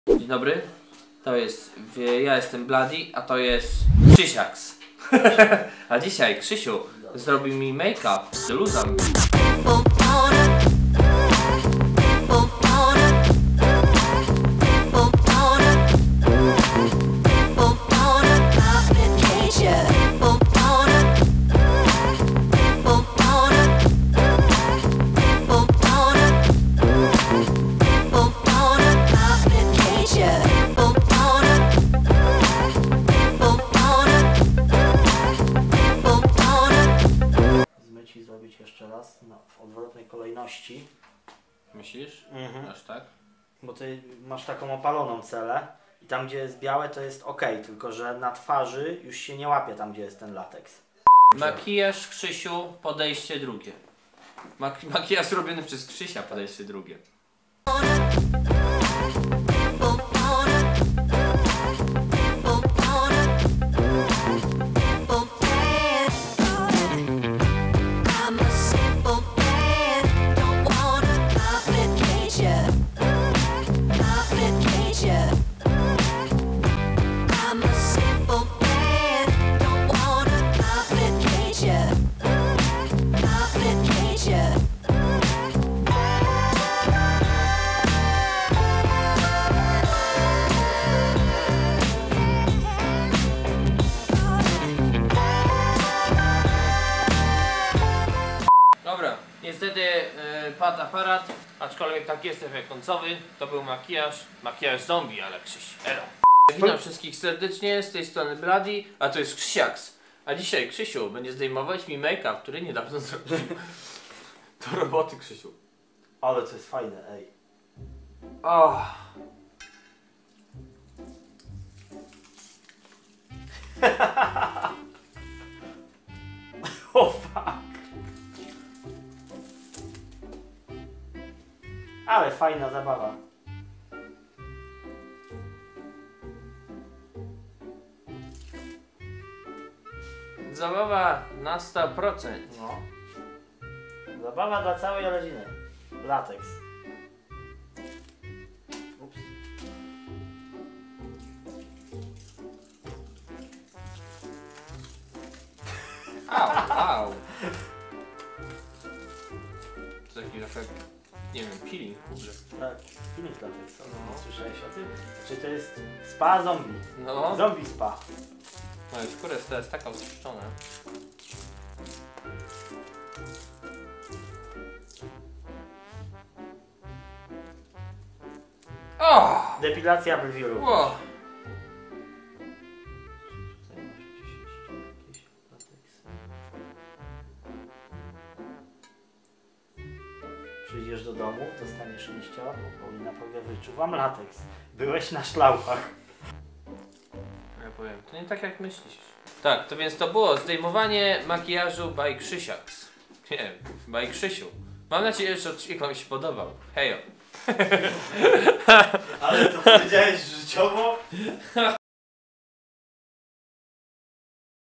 KRZYSIU ROBI MI MAKE UP W STYLU ZOMBIE
Nikt się nie spodziewał, że Krzysiu zrobi mi taki make up ;P